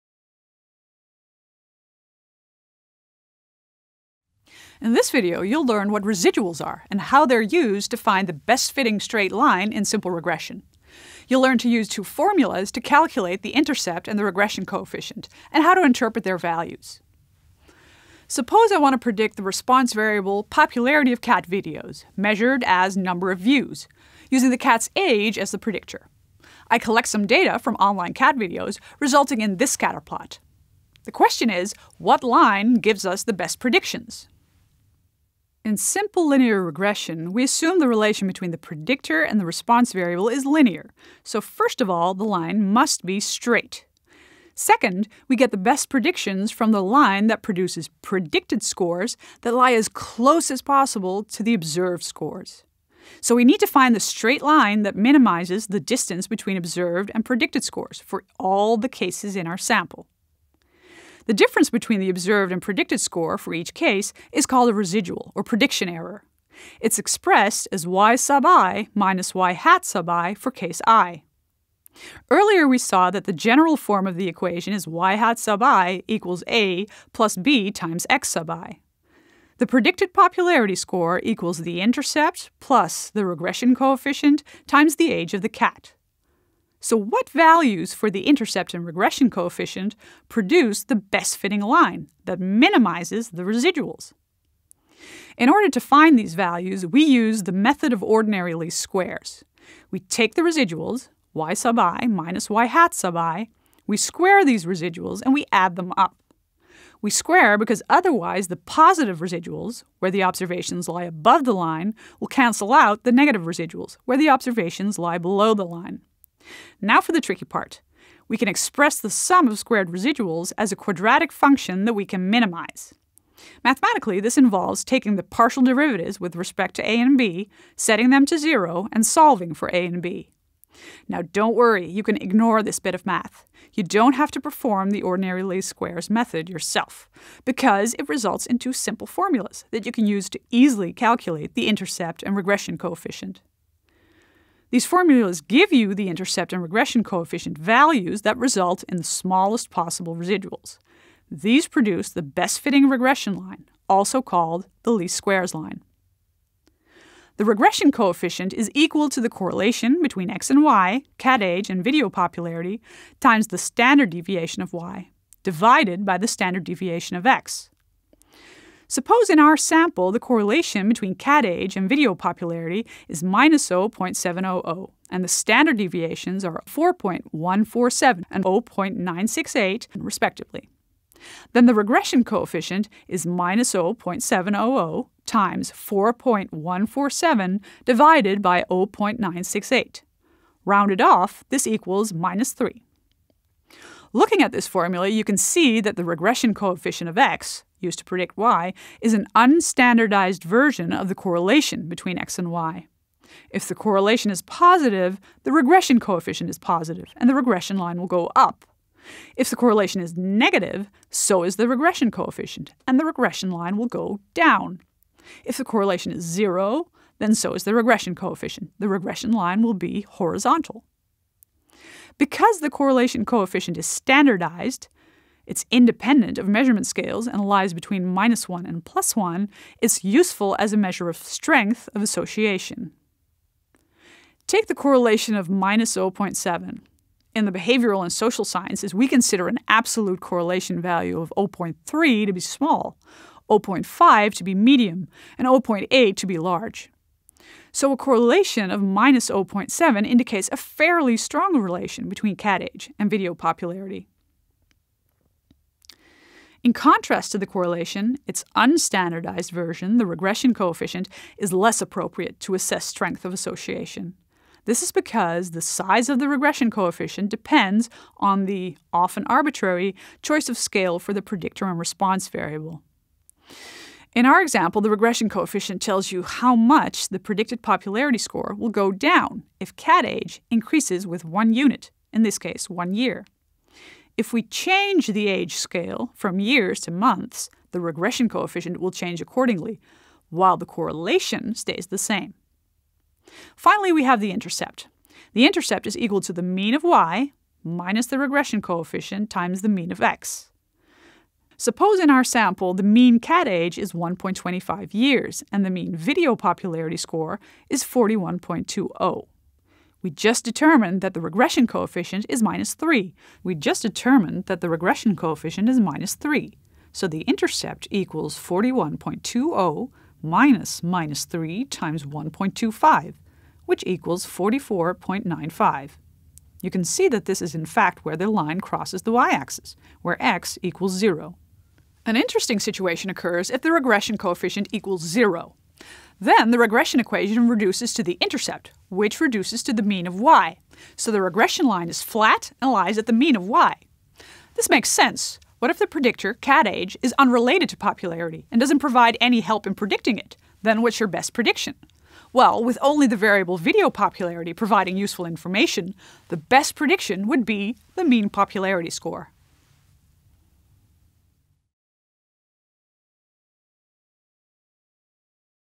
3.2 The regression equation | Inferential Statistics | Simple regression | UvA
In this video you learn what residuals are and how they are used to find the best fitting straight line in simple regression. You learn to use two formulas to calculate the intercept and the regression coefficient and how to interpret their values.